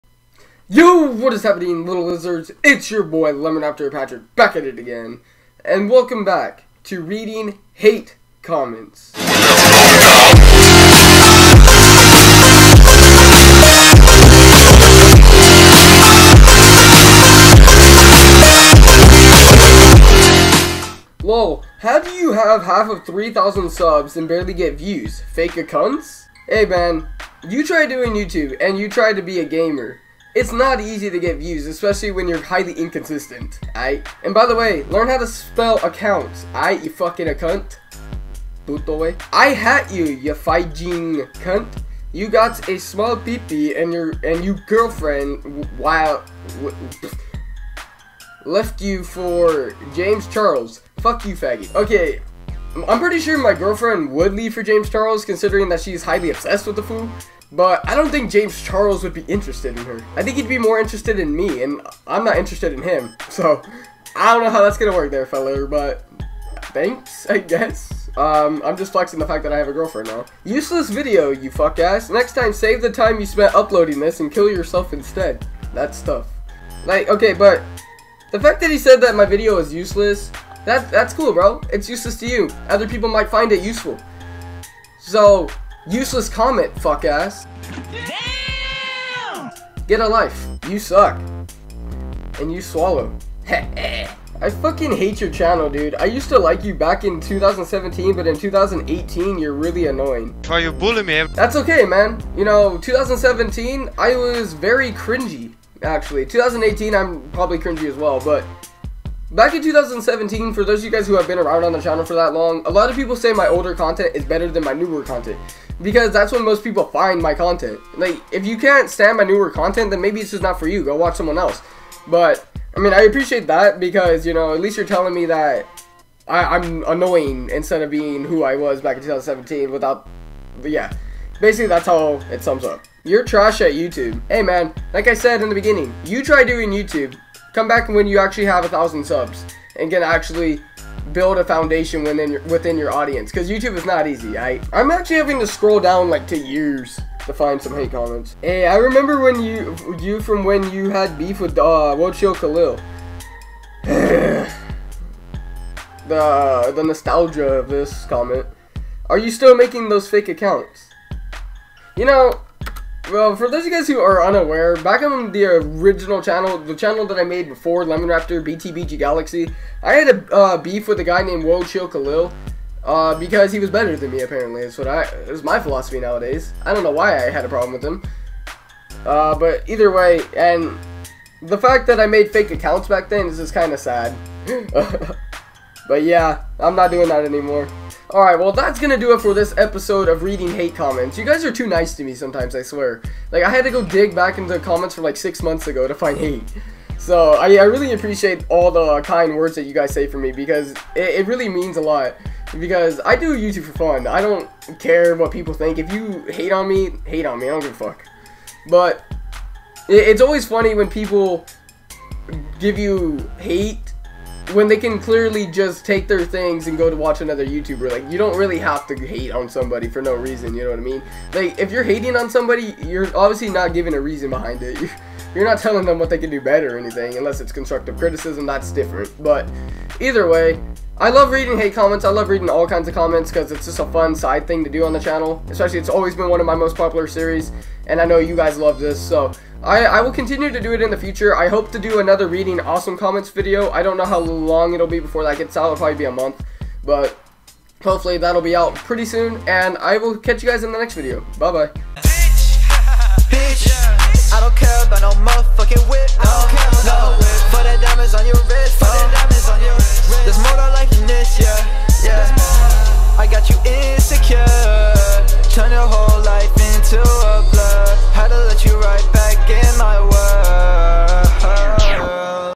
why you BULLY me!
READING HATE COMMENTS... AGAIN

Today we read youtube comments again, but not just any youtube comments, youtube hate comments. These comments were bullying me more than the people who wrote them. Its kind of sad when people give content creators a form of hatred like this, but here's LemonRaptor making fun of them. ENJOY!